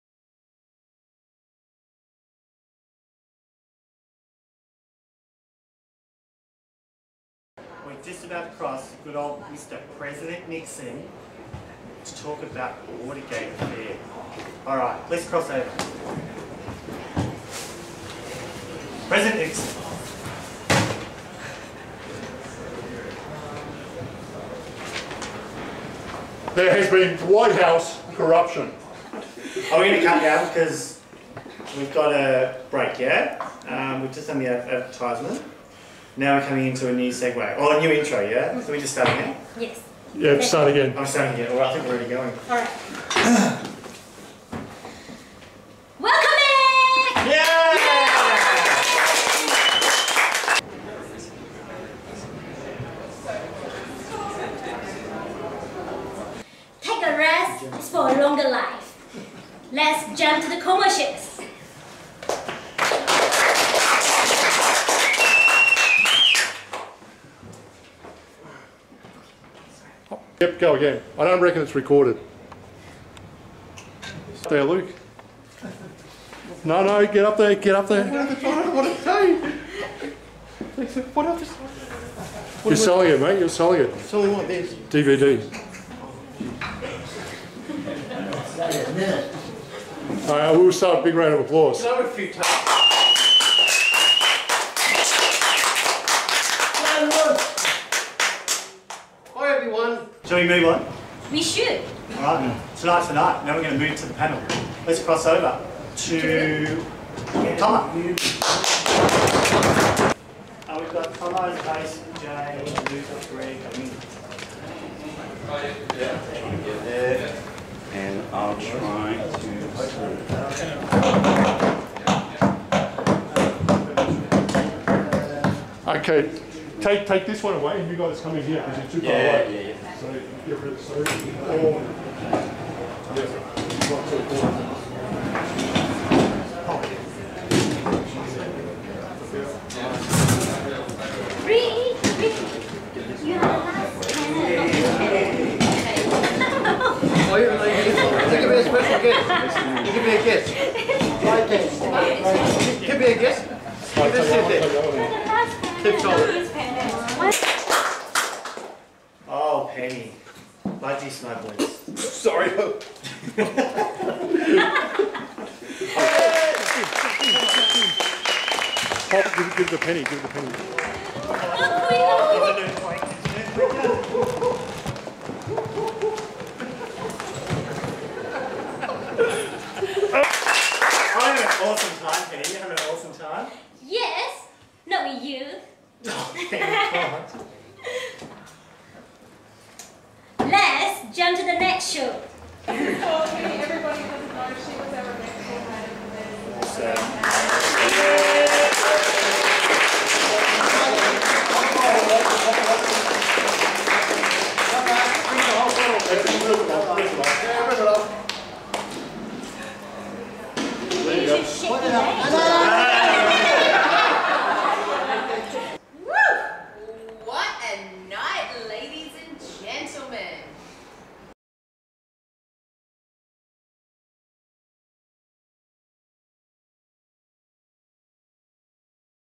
Late Night Show BLOOPERS - Toastmasters - Mighty Docklands & Smashing the Envelope
Mighty Docklands & Smashing the Envelope Toastmasters hosted this sensational event in Melbourne. Starring Penny (Smashing) and super excited Steve (Mighty)